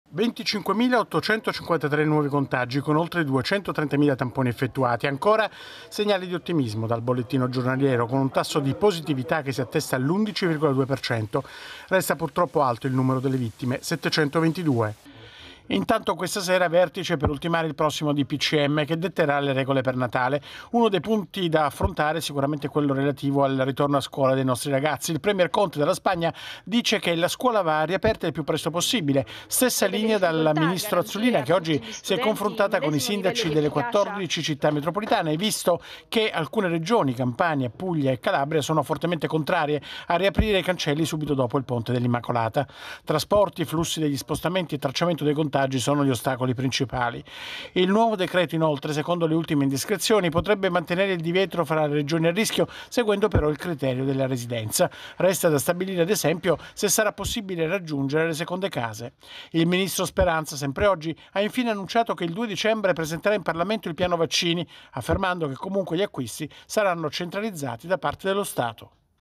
Covid-19 in Italia, sono oltre 700 le vittime nelle ultime 24 ore
Sono oltre 700 le vittime per il covid nelle ultime 24 ore. Più di 25 mila i nuovi casi. Servizio di Massimiliano Niccoli